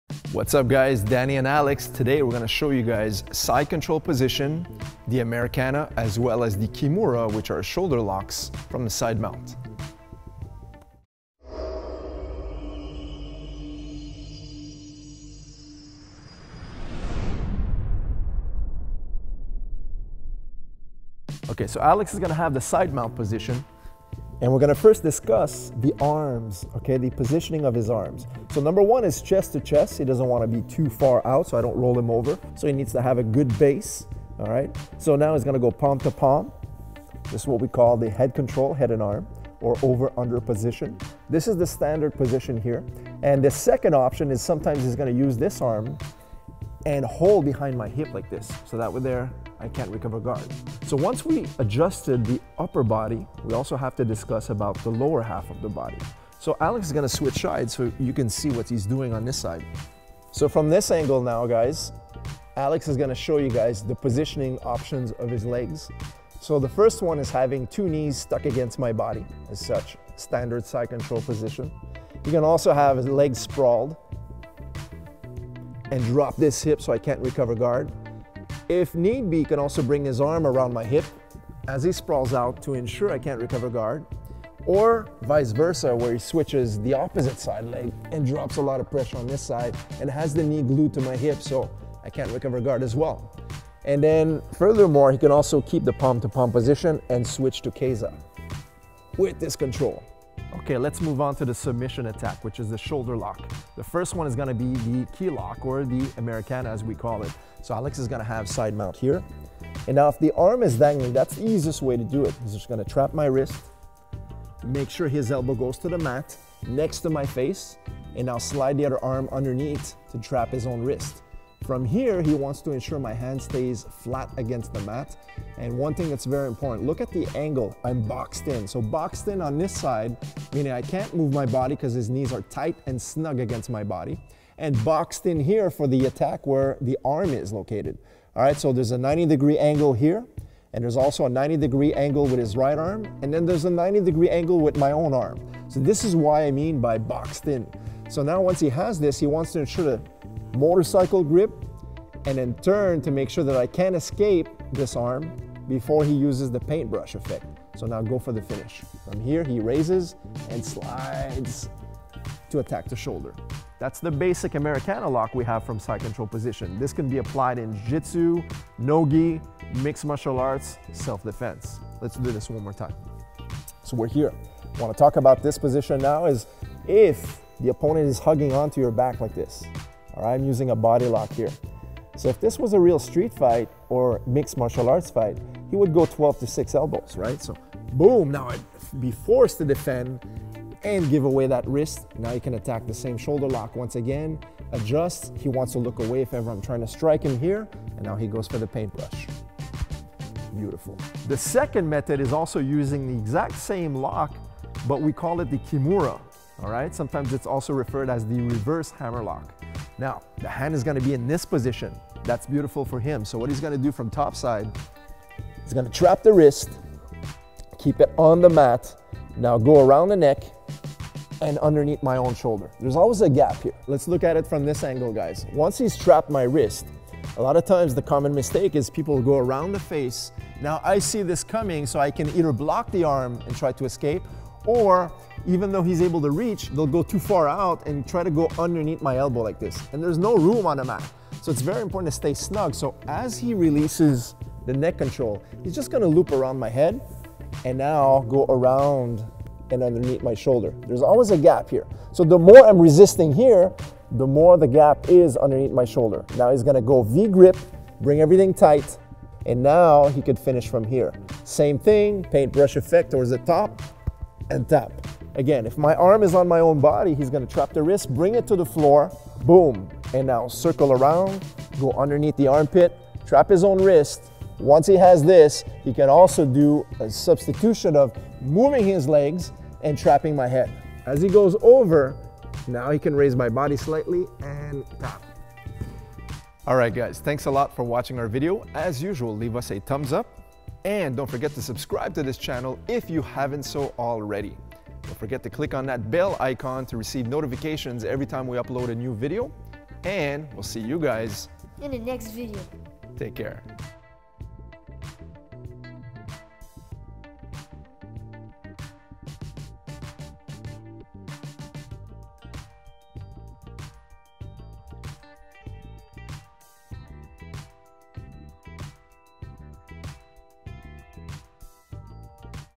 Jiu-Jitsu Fundamentals | Side Control + The Americana & Kimura
In this video segment, we take a look at the Side Control and two of its most basic attacks which can be applied from this position.

Both these submission holds target the opponent's shoulder and should be applied slowly and carefully during the practice of both these techniques.

KIDS & ADULTS